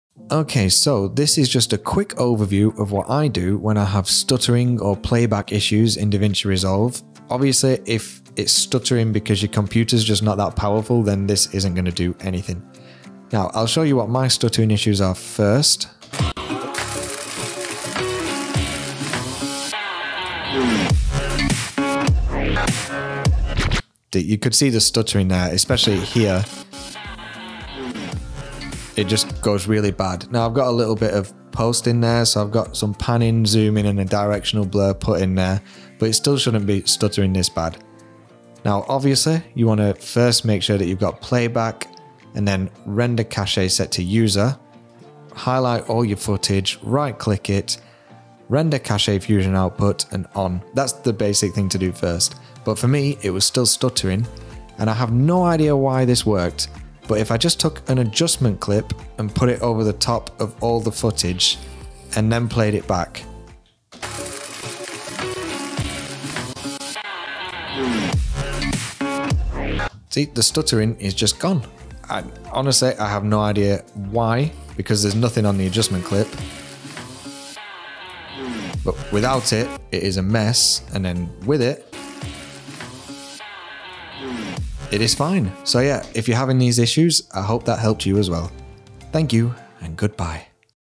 How to Fix Stuttering Footage for Smooth Playback [Davinci Resolve]
I have a good computer but Davinci Resolve seemed to struggle with playing back my footage that had effects on it even AFTER rendering.
After being annoyed by people on the forums telling me it was my computers fault, I randomly discovered this little fix so I hope this helps anyone else struggling too!

If your computer really is bad though, it won't help.